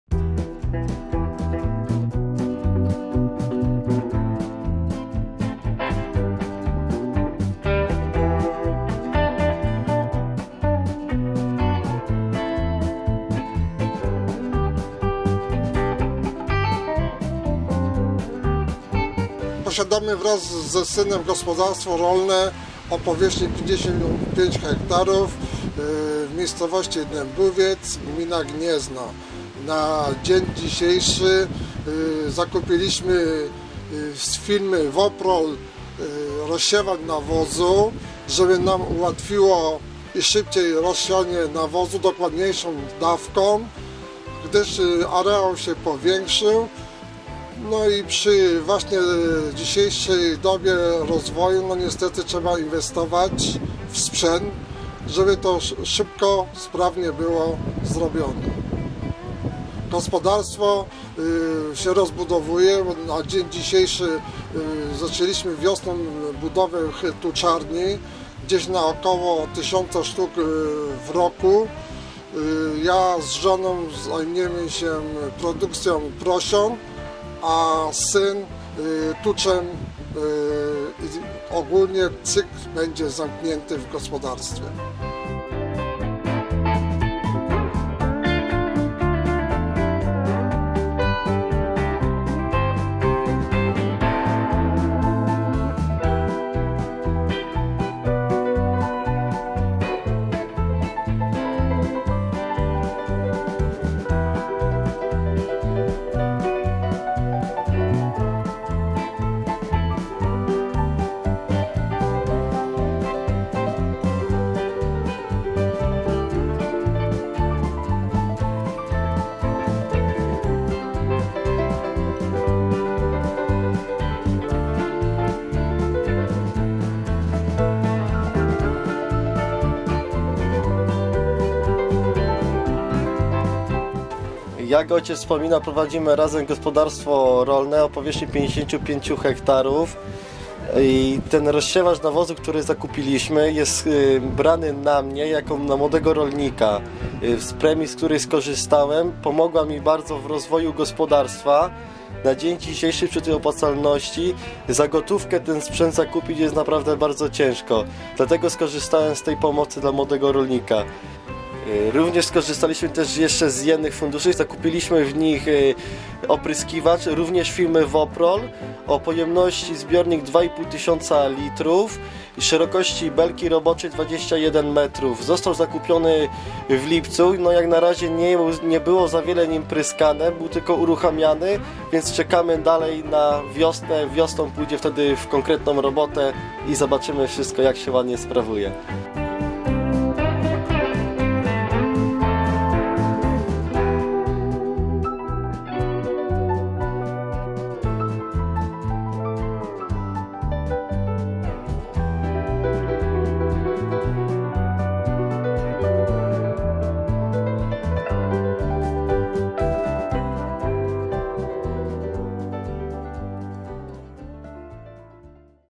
Opryskiwacz i rozsiewacz nawozów od polskiego producenta 🤔 Woprol
Gospodarstwo prowadzone przez Józefa Stachowiaka z żoną oraz syna Kamila w miejscowości Dębowiec koło Gniezna. Przed zakupem opryskiwacza i rozsiewacza nawozów Kamil szukał ciekawych maszyn i interesującej oferty m. in. na youtube. Znalazł na naszym kanale filmowym dwa filmy o opryskiwaczach Woprol i obaj panowie zdecydowali się na zakup maszyn tej marki.